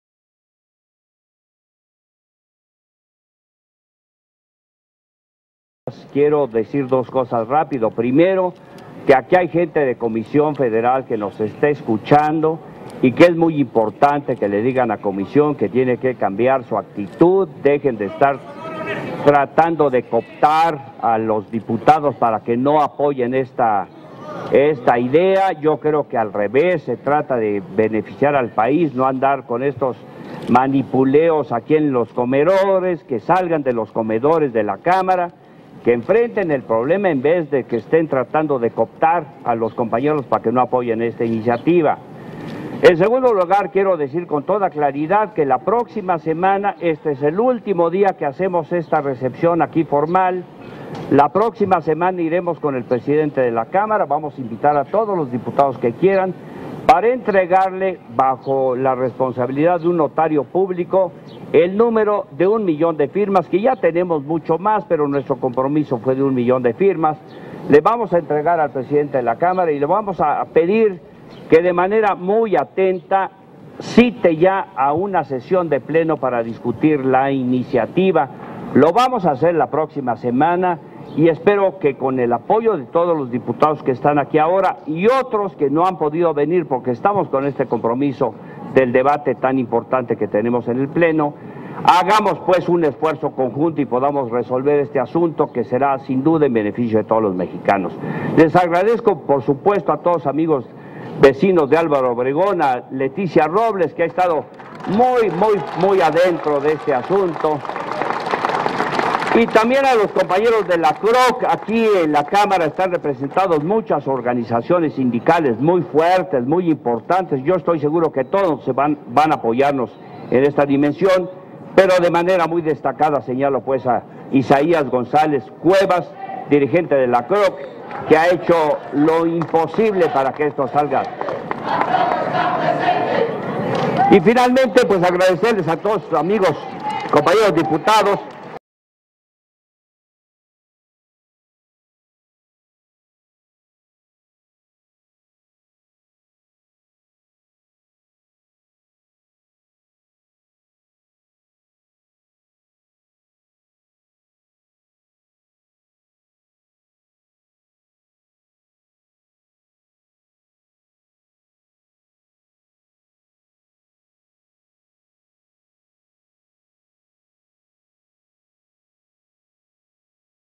Es una responsabilidad de todos frenar los abusos en la CFE diputado César Augusto Santiago
Se entrega esta semana el paquete de firmas al presidente de la Cámara de Diputados.
Debe citarse ya al debate y discusión para crear la Comisión Reguladora de Tarifas Eléctricas.
La CFE manda personal a la Cámara de Diputados para frenar este movimiento.
La Comisión Federal de Electricidad tiene que cambiar su actitud y dejar de cabildear con los diputados para que abandonen el movimiento, en lugar de ello deben tomar conciencia y hacer su parte para que se frenen los abusos en el cobro de las tarifas.
La próxima semana le vamos entregar al presidente de la Cámara de Diputados este paquete de un millón de firmas para que cite ya al debate y discusión de las reformas que son necesarias para crear la Comisión Reguladora de tarifas eléctricas.
Debemos involucrarnos todos en esto, frenar estos abusos es por el bien de todos los mexicanos y del país, esa es nuestra responsabilidad, y por ello agradezco la participación de todos los diputados involucrados en este movimiento no sólo del PRI, sino de muchos otros partidos, así como al dirigente de la CROC el diputado Isaías González Cuevas.